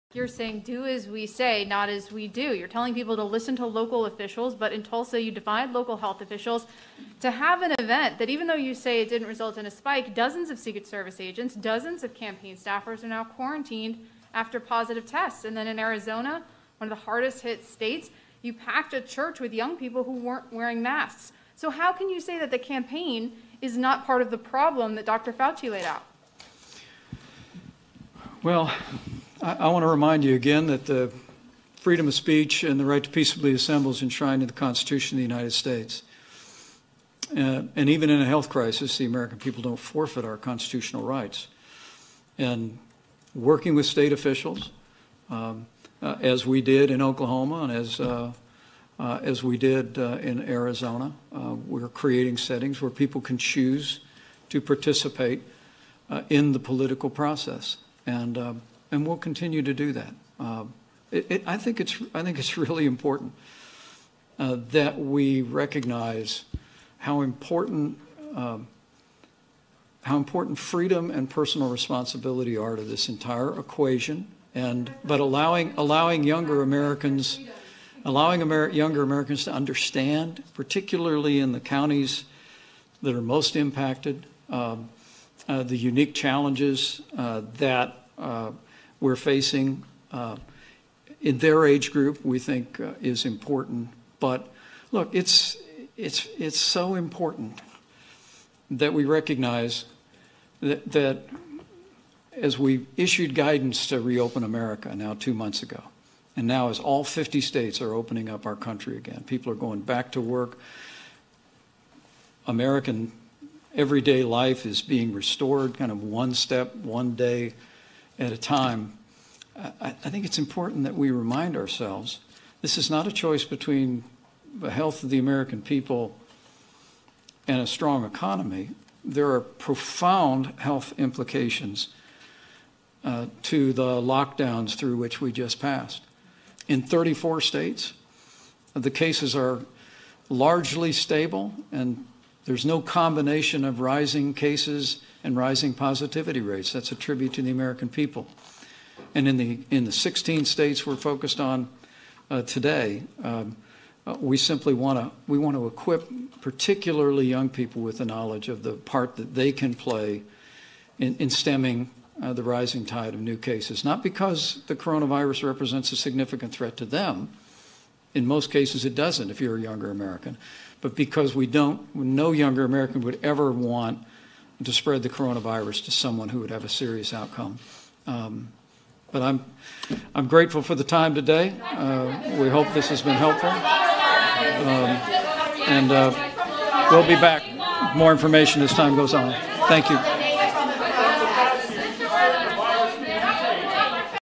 VICE PRESIDENT PENCE: AN ALTERNATIVE REALITY
THE FIRST CORONAVIRUS TASK FORCE BRIEFING IN OVER A MONTH OR MORE AND
"SAY WHAT?"